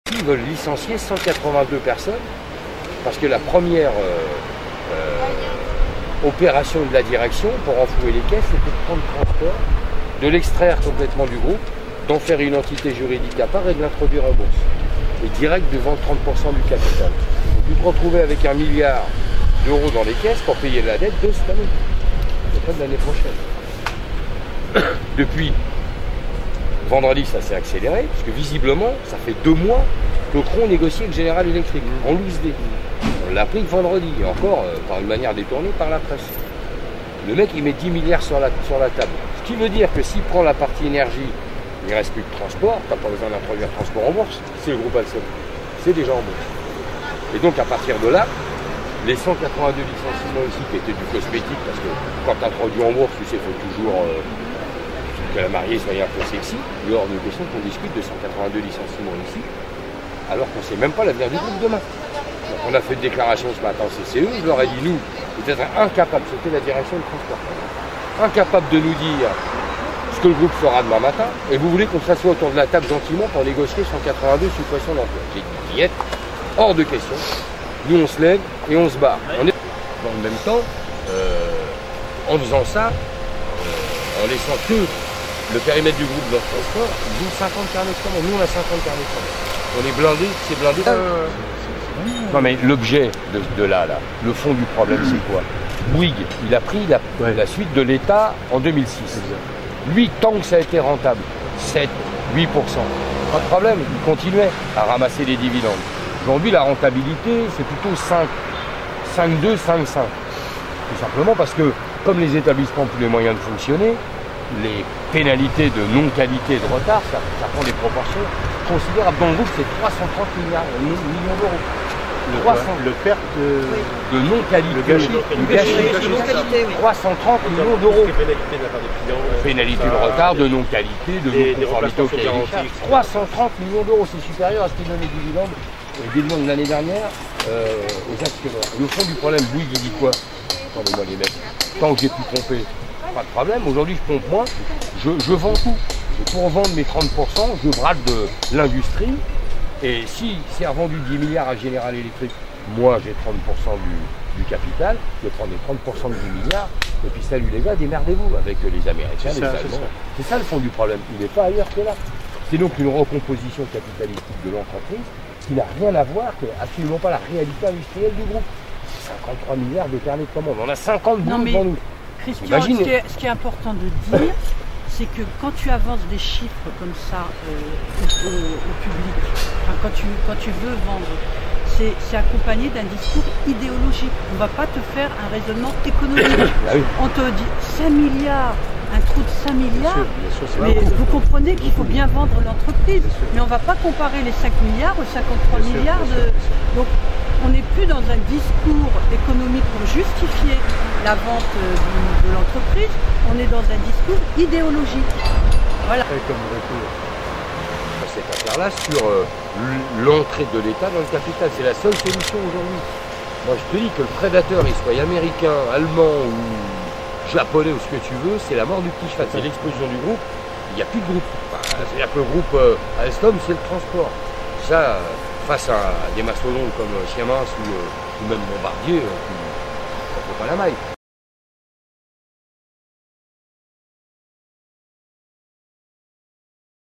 C. Garnier,représentant CGT au C. d'Alstom : "Américains ou Allemands, on ne veut pas de prédateurs"
Patrick Le Hyaric, directeur de l'Humanité, député au Parlement européen, a rencontré des élus et des responsables syndicaux d'Alstom mardi 29 avril à Saint-Ouen, au siège du groupe.
Pendant un échange de plus d'une heure, Christian Garnier - représentant CGT au Comité de groupe et délégué syndical central de la branche Transport d'Alstom - et ses camarades ont expliqué l'origine des difficultés du groupe et la solution qu'ils proposent (deuxième vidéo). Il est convaincu : "qu'ils soient Américains ou Allemands, on ne veut pas de prédateurs"

Patrick Le Hyaric a réclamé que l'État rachète la totalité des actions détenues par le groupe Bouygues. Regarder la vidéo :